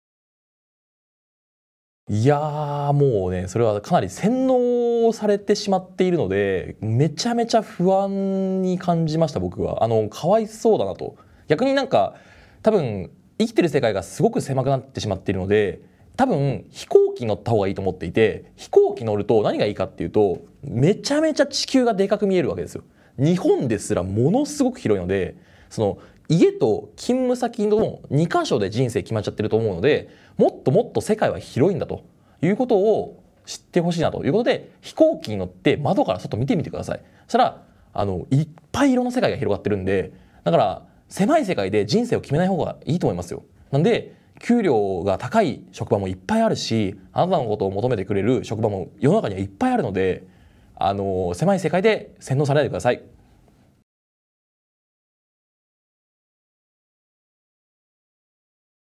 経済のプロが贈る、給料や転職に対する不安を無くす方法【行き詰まったあなたへ】
「悩むくらいなら飛行機に乗ろう、狭い世界で人生を決めない方がいい」

◆『行き詰まったあなたへ』
先の見えない世の中で様々な問題に行き詰まった時、あなたの助けになる考え方を探す。
Schoo(スクー)がおくる「頑張るキッカケを届ける」インタビュー番組です。

◆南 祐貴(みなみ ゆうき)先生
Instagram上で「毎朝午前2時過ぎに、分かりやすく日経新聞の朝刊を解説する」という「世界最速で日経新聞を解説する男（セカニチ）」のアカウントを運営。日経新聞を通じて、毎日欠かさず経済/金融を分かりやすく解説。
◆先生の代表授業はこちら 【30歳までに知っておきたい経済の知識】

 SNSでも授業情報を配信中！